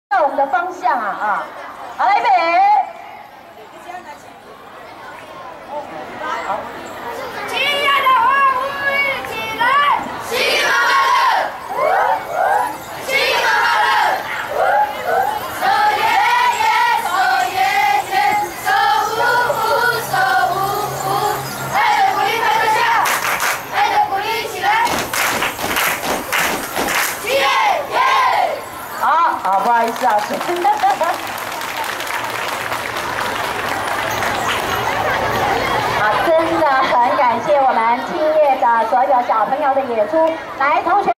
青葉國小的歡呼-青葉的歡呼一起來
台北巿建安國小和屏東青葉國小音樂交流,5/24朝會演出,他們的歡呼好可愛!